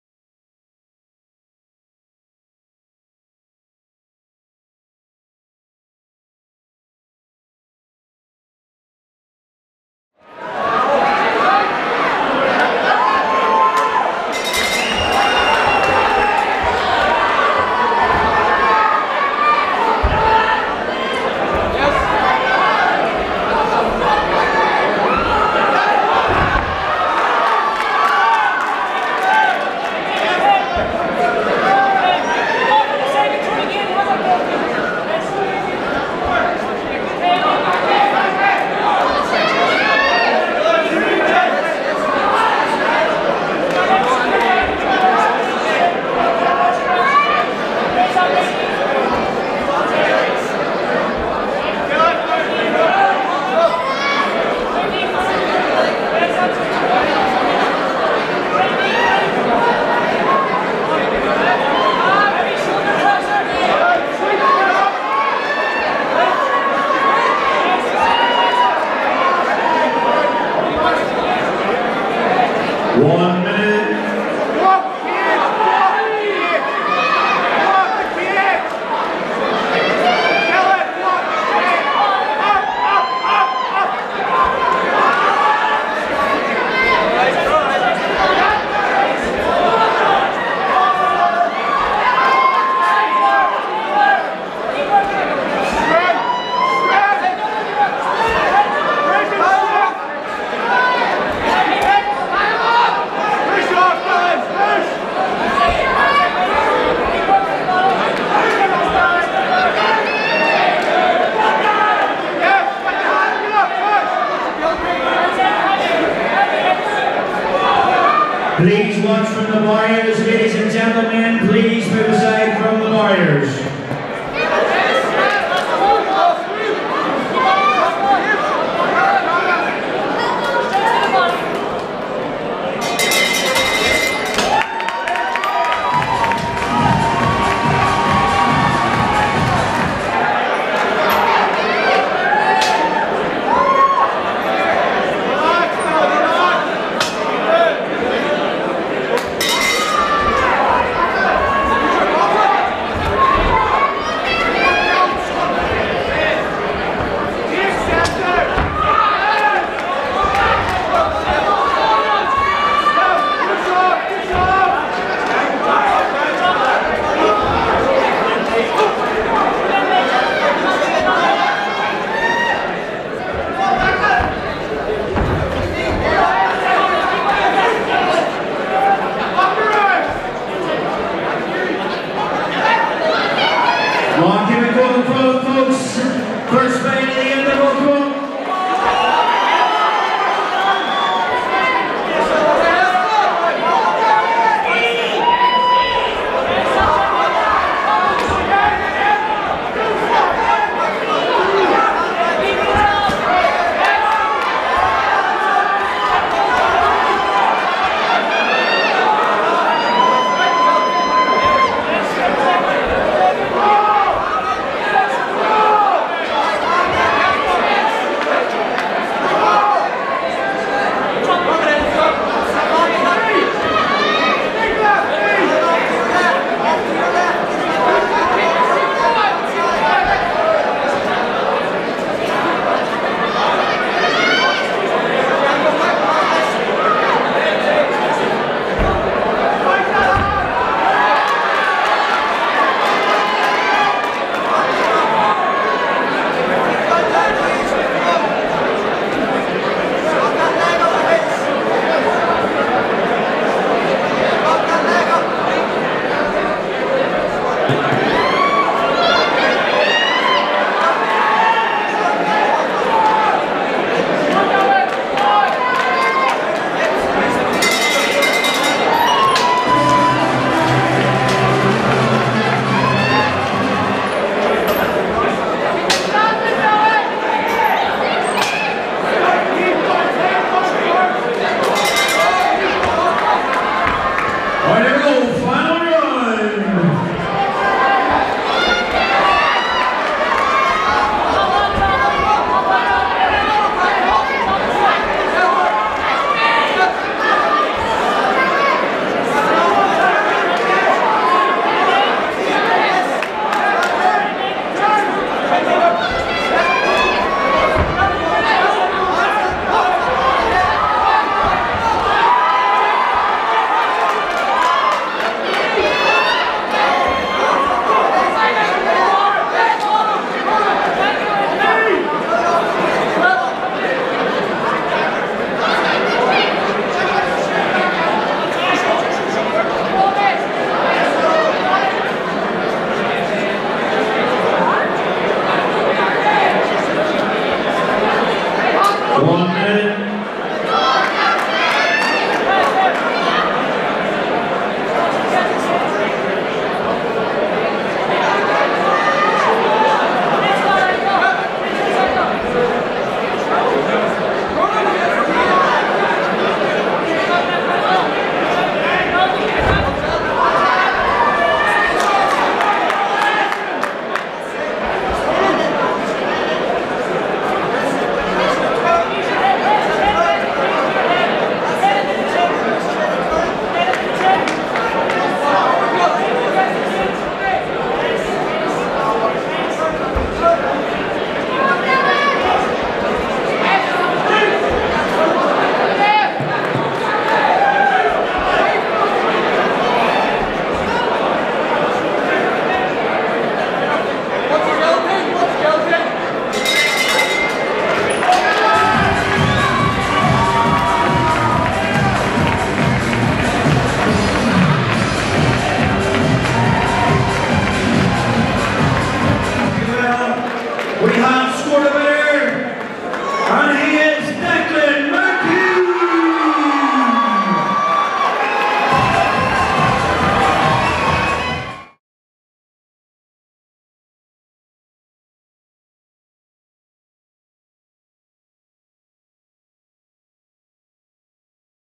Ultra MMA | Belfast | Dylan Augus VS Declan McKee
Belfast Ultra MMA 29th September 2018

8 Weeks FREE training / Get in the best shape of your life/ Raise money for Cancer Research UK